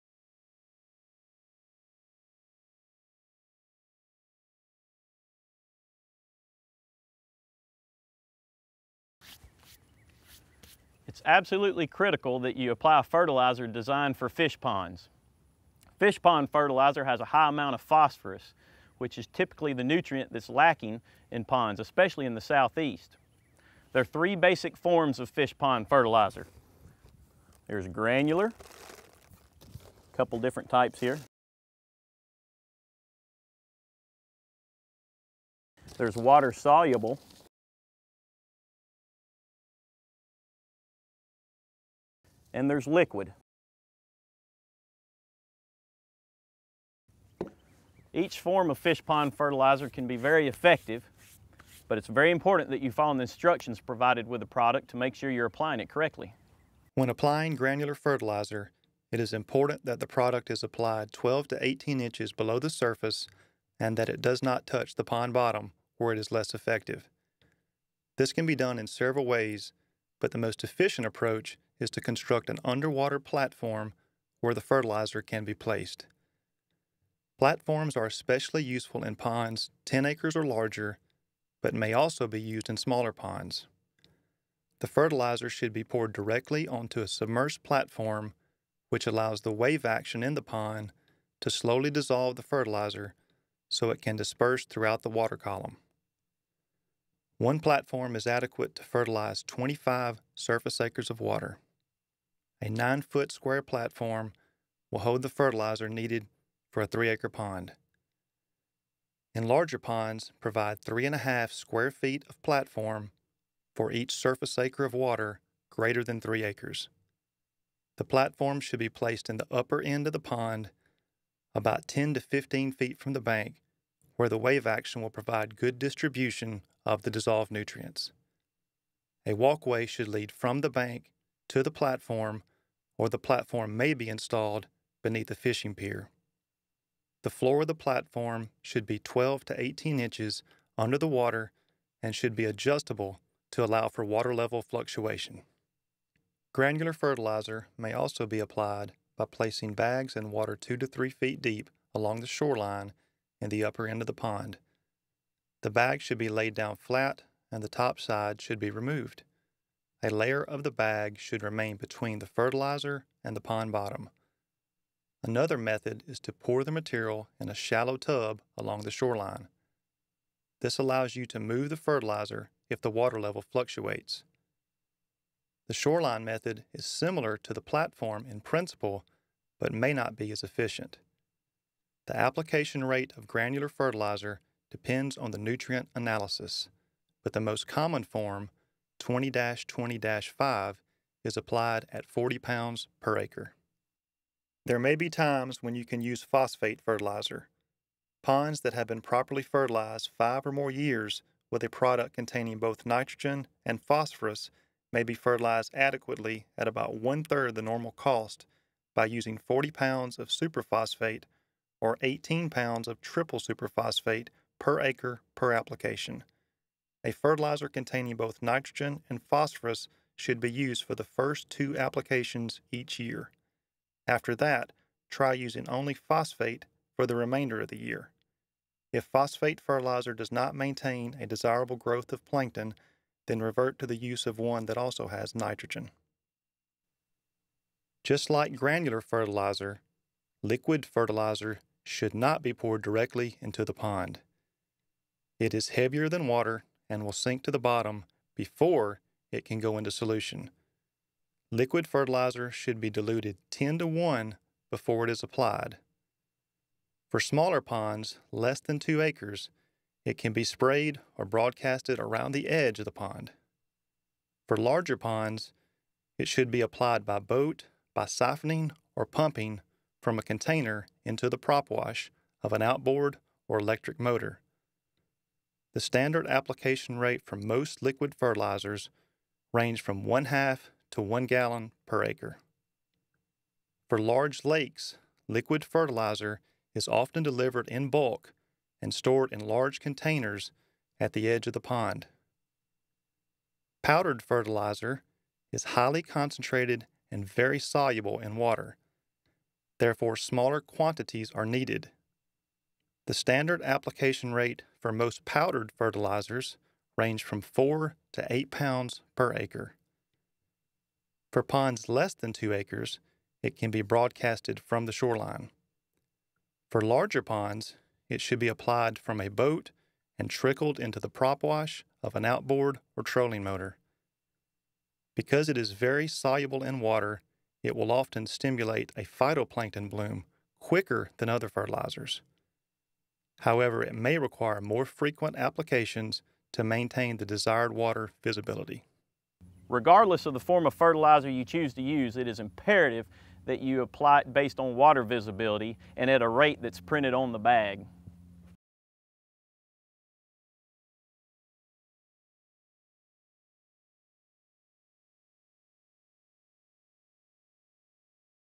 Fertilizer types & application methods
The types and application methods of fertilizers are discussed in this video.